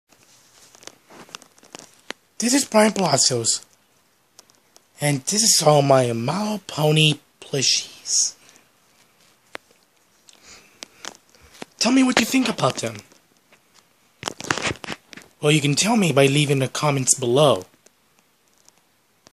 My MLP plushies
Note Tippy my Pillow Pet Tiger is not an MLP character and My black horse plush, Blue Unicorn and Brown Horse plush and also my pink zebra plush is not one of MLP characters but I still love them also My sadness plush is not an MLP character she os an Inside Out character